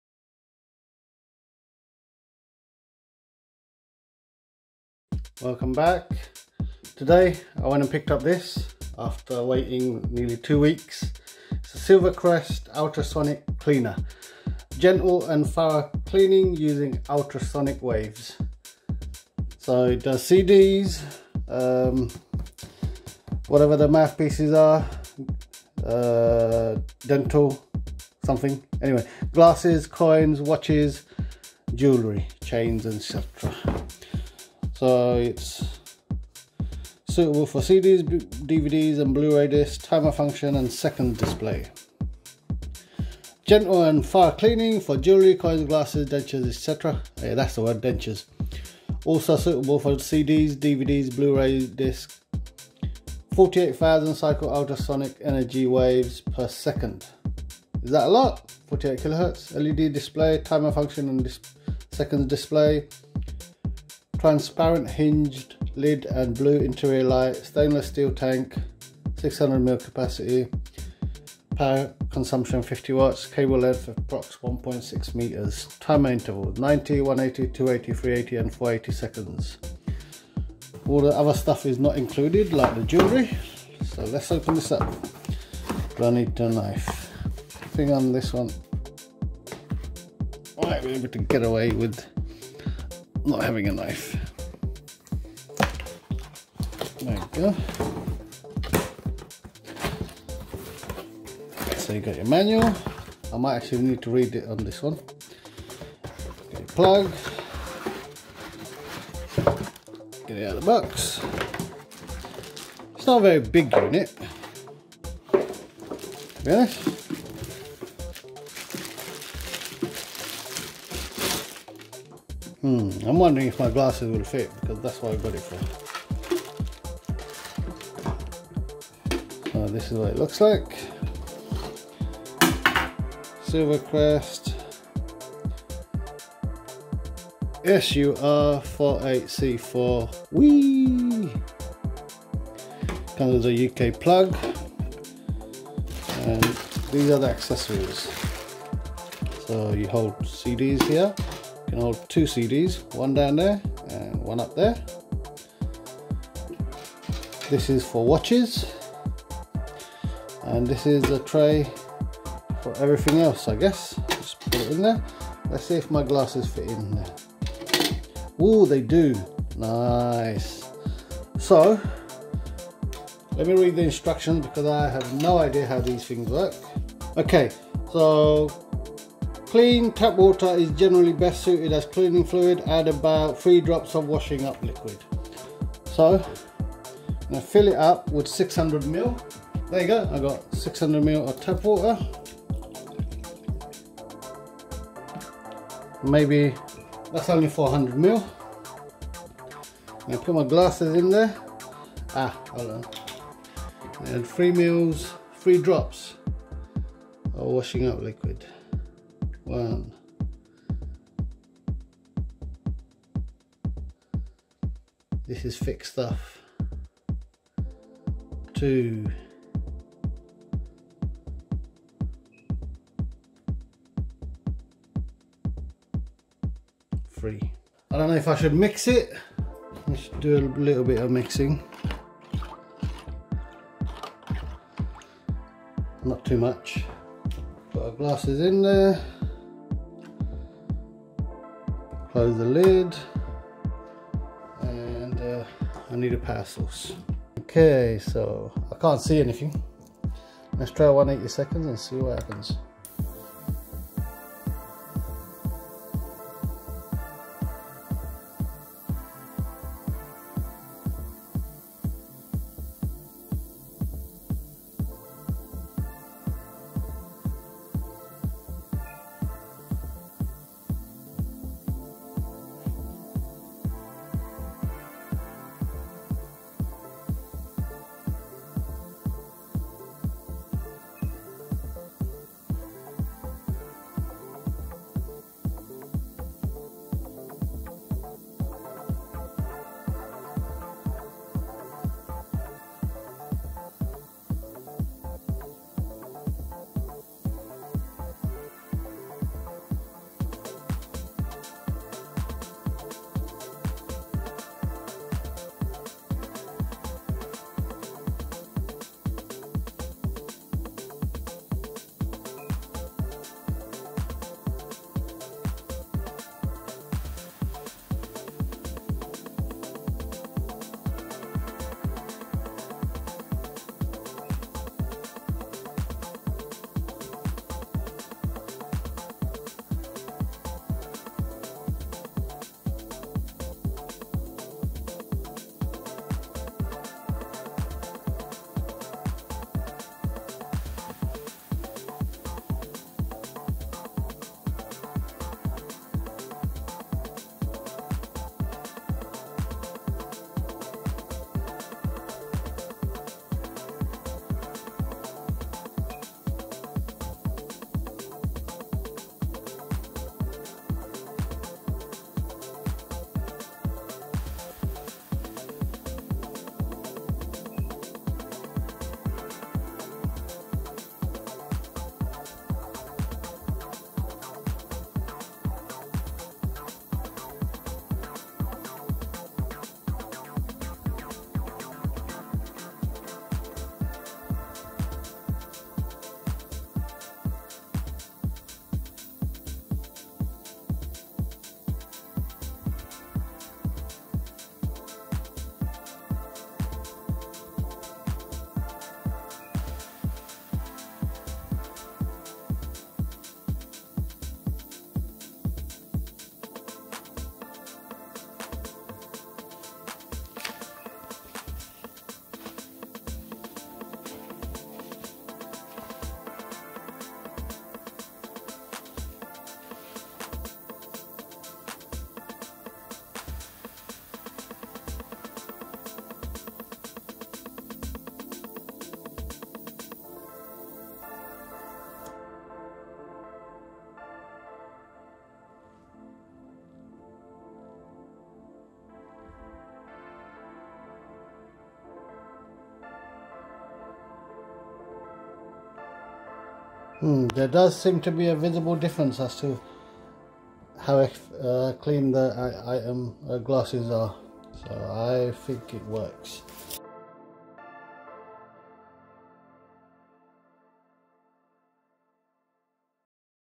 Silvercrest Ultrasonic Cleaner For Coins Jewellery Glasses CD DVD Blu Ray Discs etc - unboxing
Silvercrest Ultrasonic Cleaner For Coins Jewellery Glasses CD DVD Blu Ray Discs etc

Gentle and thorough cleaning
For jewellery, coins, glasses, dentures, and more.
Also suitable for CDs, DVDs and Blu-ray discs
48,000 cycle ultrasonic energy waves per second (cleaning frequency: 48kHz)
LED display
Timer function and seconds display
Transparent hinged lid and blue interior light
Stainless steel tank with capacity markings
Capacity: 600ml
Accessories: plastic basket, domed watch holder, holder for CDs/DVDs/Blu-ray discs
Power consumption: 50W
Cable length: 1.6m
Timer intervals: 90, 180, 280, 380 and 480 seconds
3-year warranty

£19.99 at LiDL, I got £2.00 off with a voucher

Ultrasonic cleaning is a process that uses ultrasound (usually from 20 to 40 kHz) to agitate a fluid, with a cleaning effect. Ultrasonic cleaners come in a variety of sizes, from small desktop units with a capacity of less than 0.5 litres (0.13 US gal), to large industrial units with a capacity approaching 1,000 litres (260 US gal).

The principle of the ultrasonic cleaning machine[1] is to convert the sound energy of the ultrasonic frequency source into mechanical vibration through the transducer. The vibration generated by the ultrasonic wave is transmitted to the cleaning liquid through the cleaning tank wall, so that the micro-bubbles in the liquid in the tank can keep vibrating under the action of the sound wave, destroying and separating the dirty adsorption on the surface of the object.

Depending on the object being cleaned, the process can be very rapid, completely cleaning a soiled item in minutes. In other instances cleaning can be slower, and exceed 30 minutes.[2]

History
Ultrasonic cleaning has been used industrially for decades, particularly to clean small intricate parts, and to accelerate surface treatment processes.[4]

It appears that ultrasonic cleaners developed as a natural evolution of several earlier inventions that used vibrations to agitate and mix substances, and thus there is no clear "inventor" of ultrasonic cleaning. US patent 2815193, issued December 1954, is the earliest patent on record that specifically uses the term "Ultrasonic cleaning" although earlier patients refer to use of ultrasound for "intense agitation," "treatment" and "polishing," e.g. US 2651148.

By the mid-1950s there were at least three ultrasonic cleaner manufacturers established in the United States and two in the United Kingdom; and by the 1970s ultrasonic cleaners were widely established for industrial and domestic use.[5][6]

Uses
Most hard, non-absorbent materials (metals, plastics, etc.) not chemically attacked by the cleaning fluid are suitable for ultrasonic cleaning. Ideal materials for ultrasonic cleaning include small electronic parts, cables, rods, wires and detailed items, as well as objects made of glass, plastic, aluminium or ceramic.[11]

Ultrasonic cleaning does not sterilize the objects being cleaned, because spores and viruses will remain on the objects after cleaning. In medical applications, sterilization normally follows ultrasonic cleaning as a separate step.[12]

Industrial ultrasonic cleaners are used in the automotive, sporting, printing, marine, medical, pharmaceutical, electroplating, disk drive components, engineering and weapons industries.

Ultrasonic cleaning is used to remove contamination from industrial process equipment such as pipes and heat exchangers.

Limitations
Ultrasonic cleaning is used widely to remove flux residue from soldered circuit boards. However, some electronic components, notably MEMS devices such as gyroscopes, accelerometers and microphones can become damaged or destroyed by the high intensity vibrations they are subjected to during cleaning. Piezoelectric buzzers can work in reverse and produce voltage, which may pose a danger to their drive circuits.